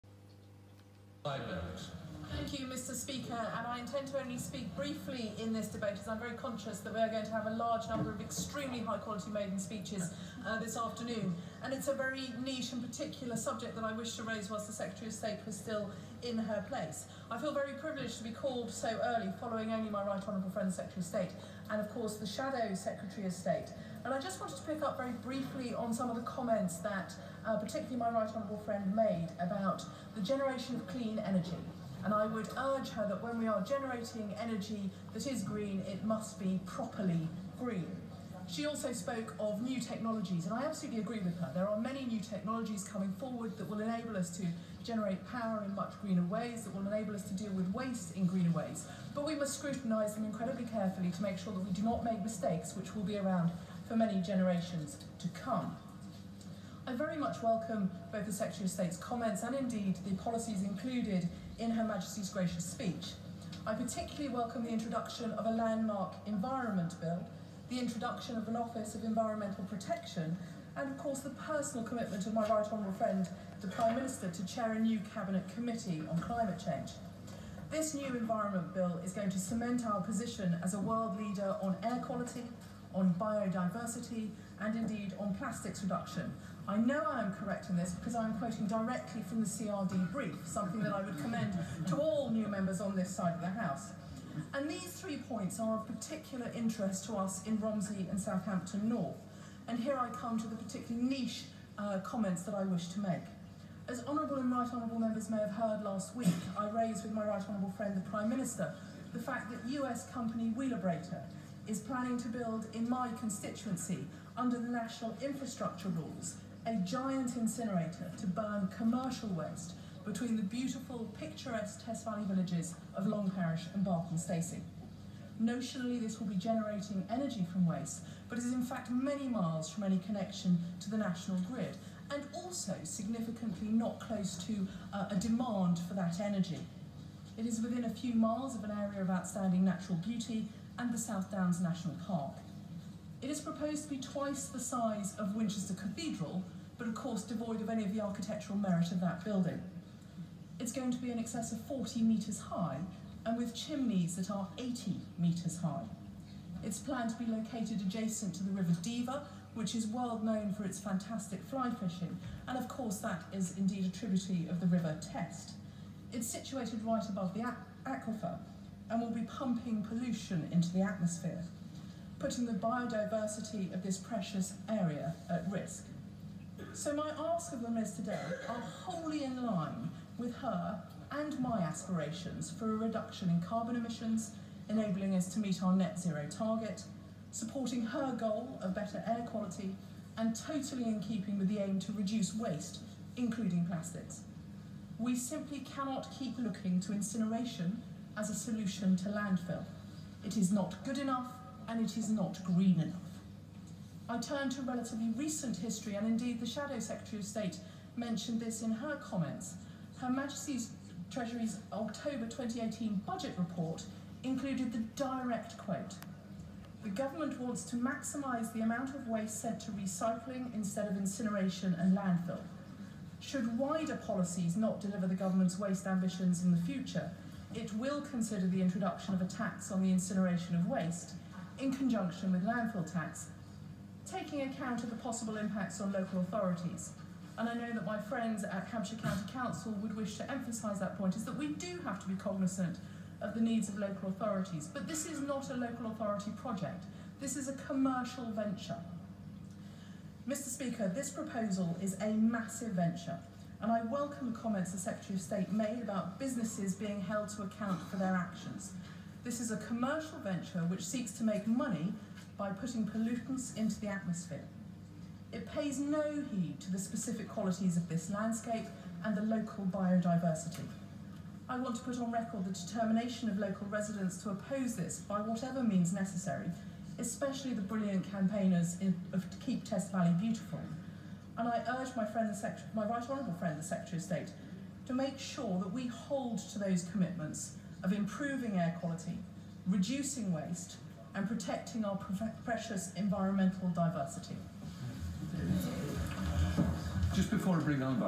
Caroline Noakes MP Romsey and Southampton North: Harewood Incinerator proposal, Longparish, Andover
Queen’s Speech: Green Energy/Carbon Neutral by 2050 House of Commons 15/01/2020